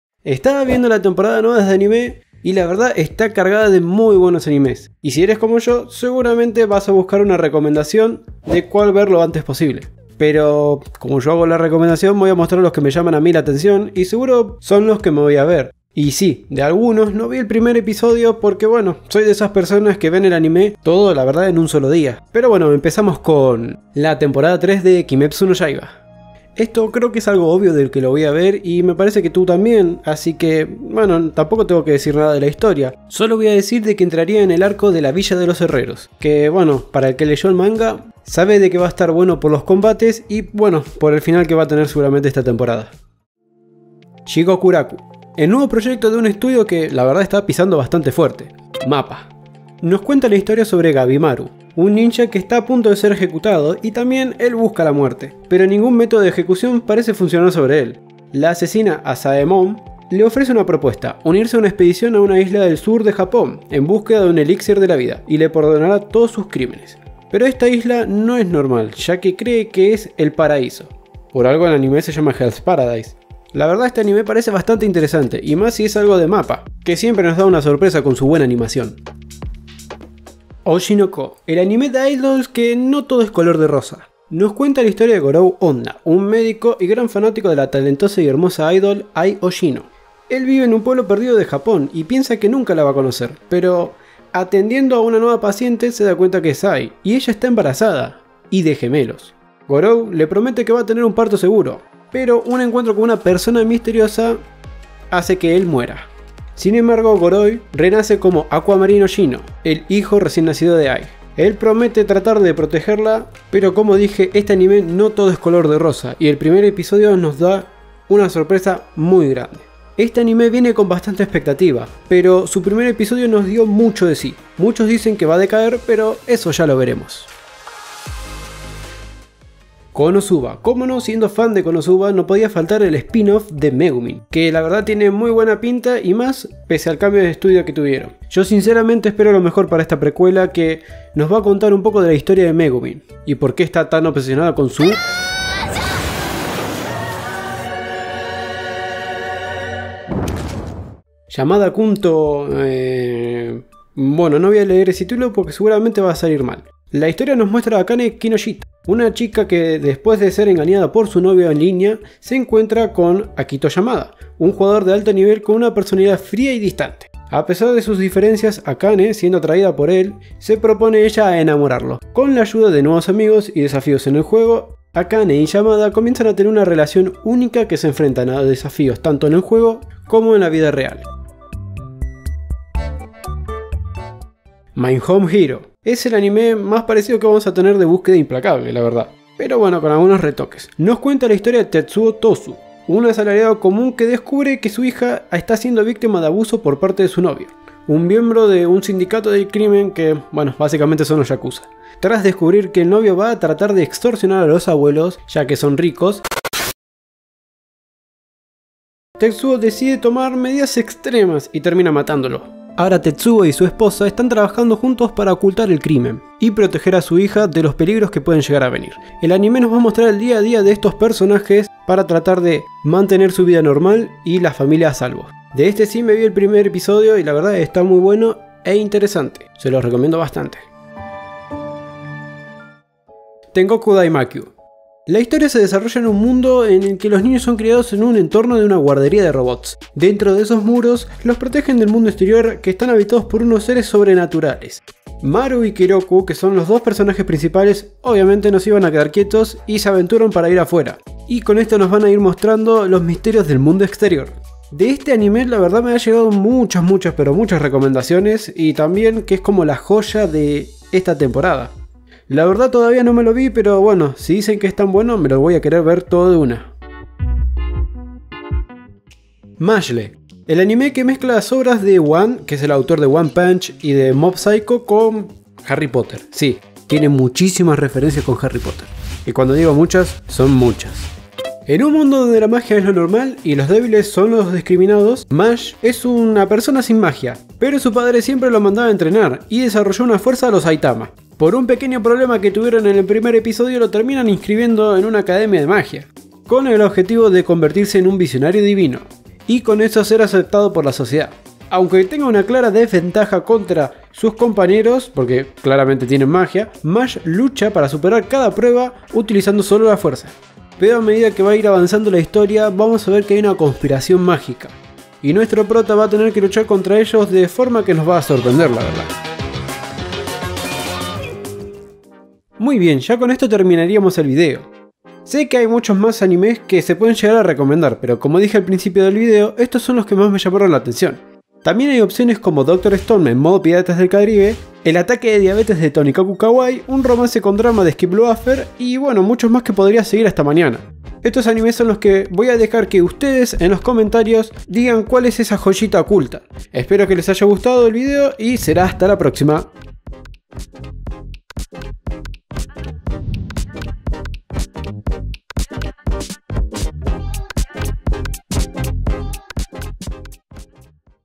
♦ ANIMES DE LA TEMPORADA QUE TIENES QUE VER ♦ | Recomendaciones de Temporada de PRIMAVERA 2023
La temporada de anime de primavera 2023 ha estado a con todo durante las últimas semanas, y todos los fanáticos del anime se preguntan: "¿Qué anime debería ver en esta temporada?". Aca vamos a hacer un pequeño top de los mejores animes de la temporada de primavera 2023, basado en mis recomendaciones y mis gustos

En este top, vas a encontrar una selección cuidadosamente elegida de los animes más emocionantes, divertidos y atractivos de esta temporada. (Re formal lo dejaba xD) Desde aventuras hasta dramas, comedias, y bastante acción hay algo para todos los gustos en esta lista.

Estos son solo algunos animes que recomendamos de la temporada de primavera 2023. Si eres un fan del anime o simplemente estás buscando una nueva serie para ver, asegúrate de ver este video para encontrar tu próximo anime